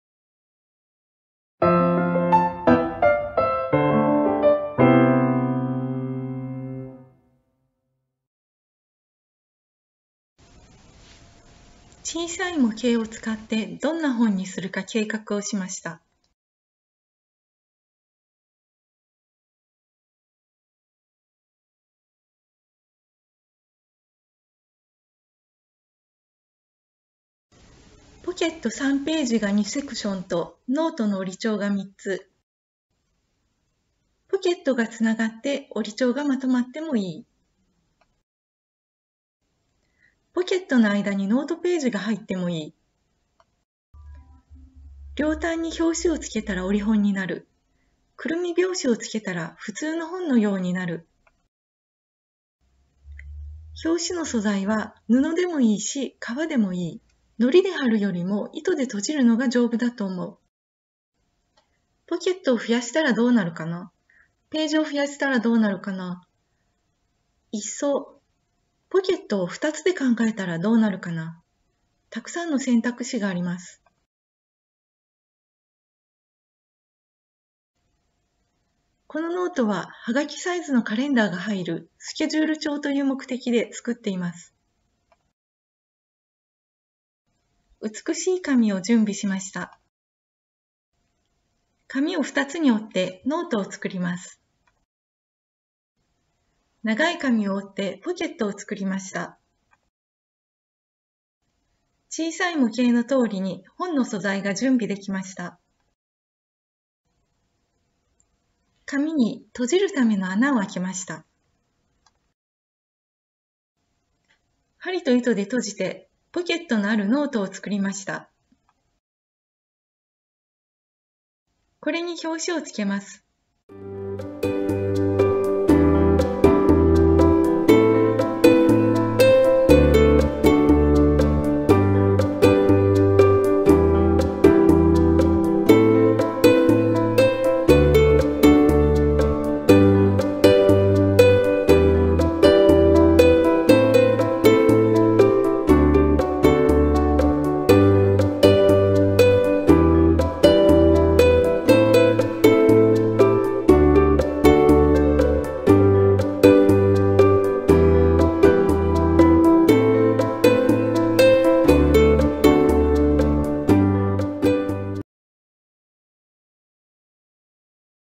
How to plan the structure of a notebook folder with pockets ポケットのあるノートフォルダの構造計画の方法
Even the structure of a book that looks complicated can be planned using a miniature model.
複雑に見える本の構造も、ミニチュアモデルを使って計画すれば、思いのままに。